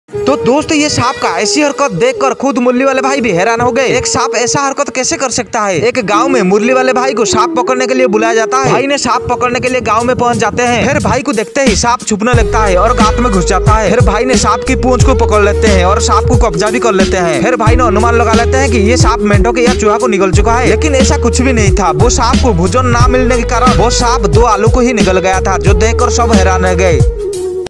सांप की ऐसी हरकत देखकर खुद मुरली वाले 🙆 भाई भी रह गई हैरान ..🤫😂
Credit by -
Murliwale Hausla